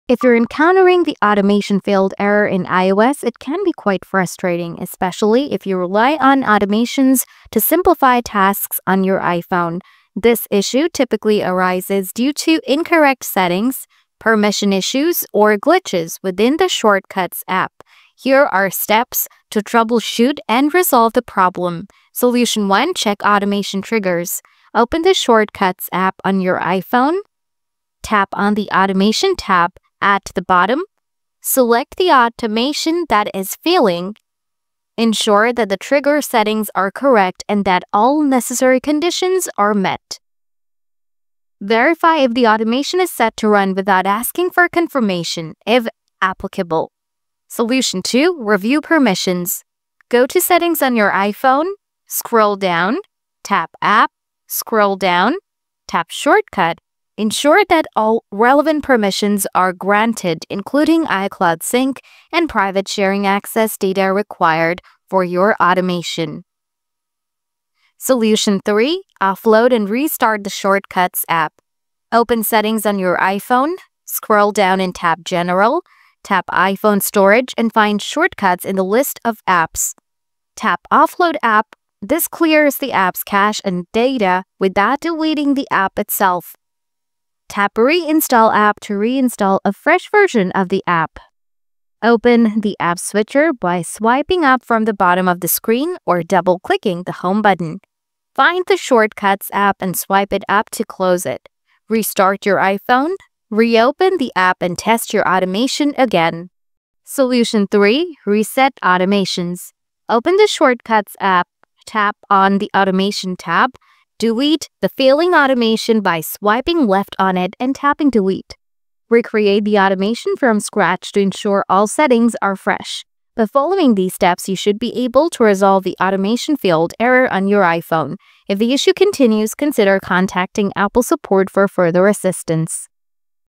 How to Fix Automation Failed Error on iPhone | Automation Not Working
Facing the "Automation Failed" error on your iPhone? Don’t worry! In this guide, we’ll help you troubleshoot and fix automation not working on your iPhone. Whether it’s an issue with the Shortcuts app, iOS settings, or automation triggers, you’ll learn how to resolve these errors step by step. Discover tips to reset or reconfigure your automations so they run smoothly without interruptions. Perfect for anyone who relies on automation to simplify tasks on their iPhone!